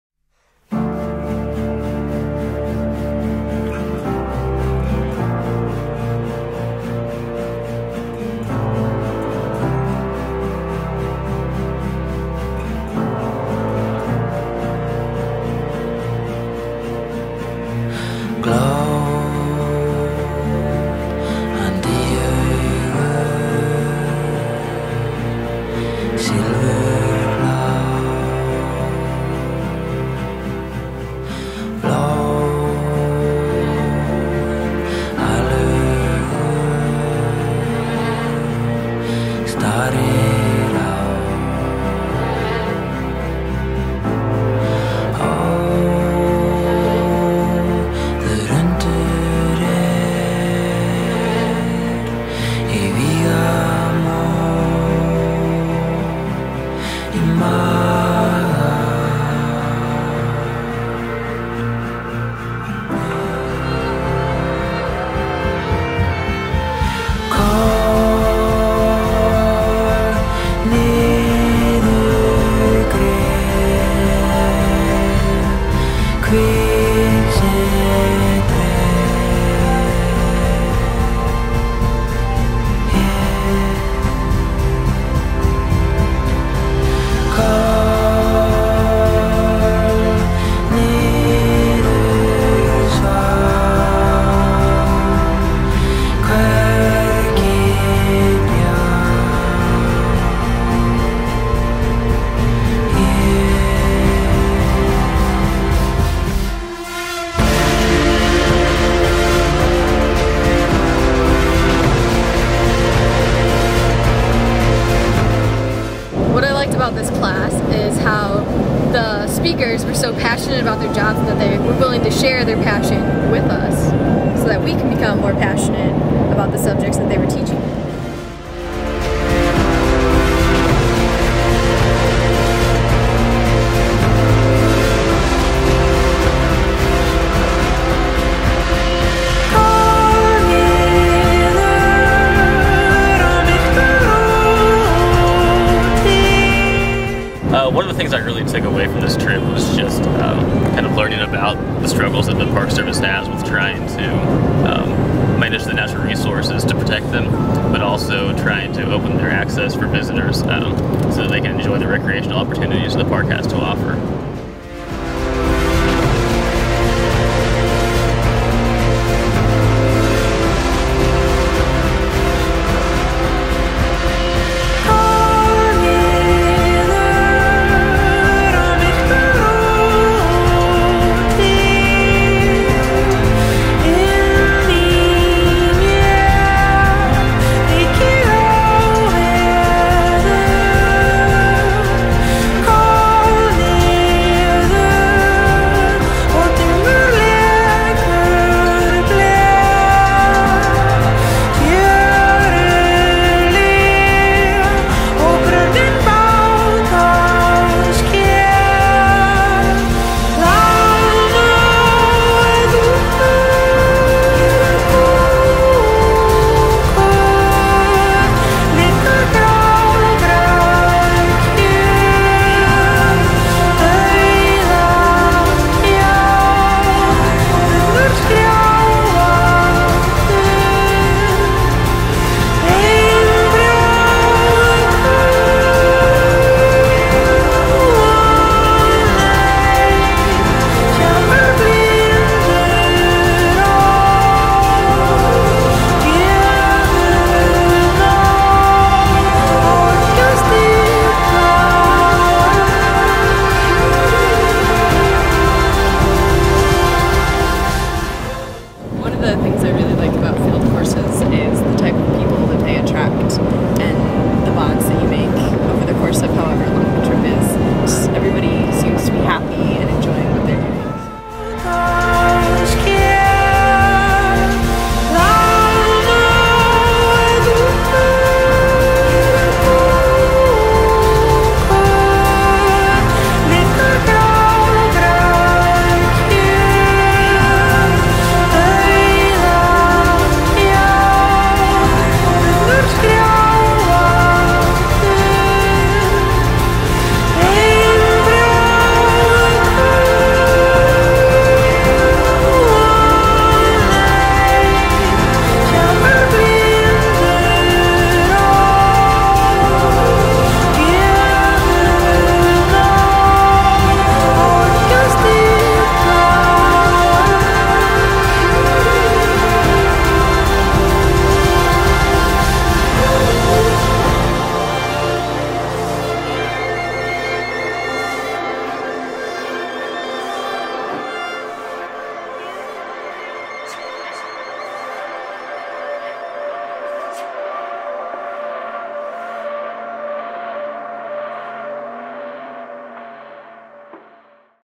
Yellowstone Field Studies 2012 | By Clinton Begley
The Wilderness Institute in cooperation with The University of Montana's College of Forestry and Conservation offered this field studies course over four days in Yellowstone National Park in February of 2012.

For more information about programs offered by the Wilderness Institute, visit http;//

A handful of still Photos from the trip may be viewed